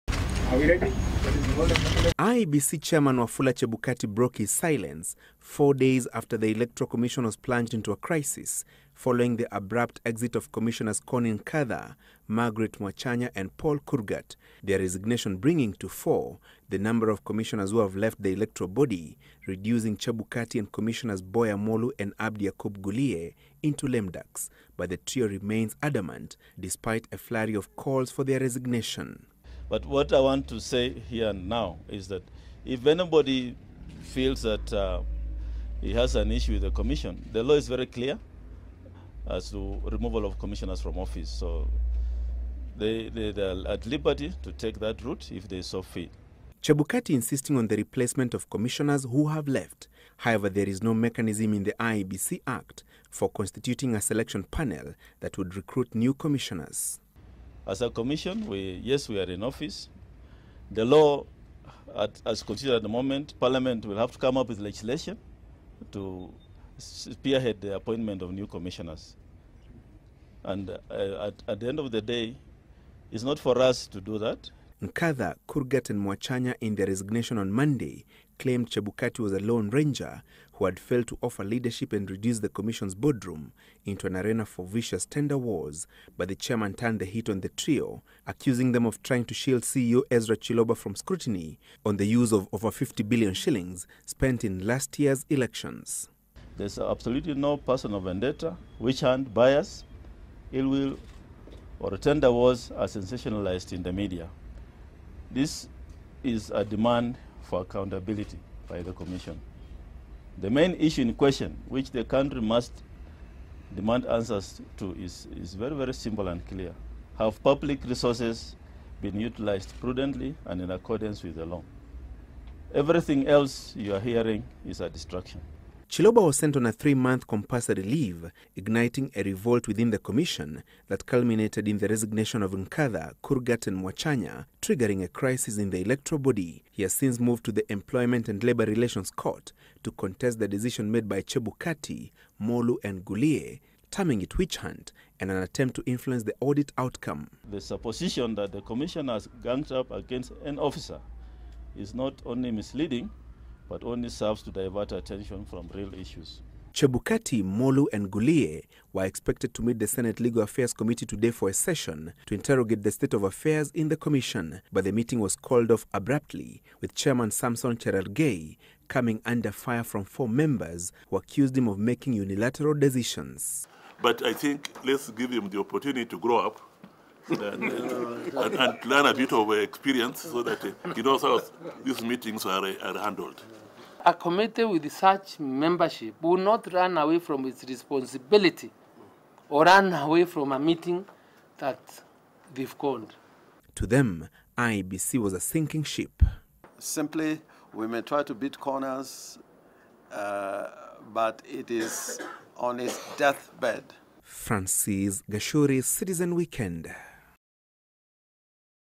Embattled IEBC chair Chebukati fights back
IEBC chairman Wafula Chebukati and two Commissioners Boya Molu and Abdi Yakub Guliye have dismissed calls for their resignation and dared anyone keen on their ouster to institute their removal through a tribunal. Chebukati says he was yet to be formally informed about the resignation of his deputy Connie Nkatha and Commissioners Margaret Mwachanya and Paul Kurgat, but asked parliament to hasten the enactment of a law, that will create a mechanism for filling the four vacancies at the commission. But as Francis gachuri reports, a senate legal affairs committee meeting with Chebukati was abruptly called off, setting its chairman Samson Cherargey on a collision course with four members.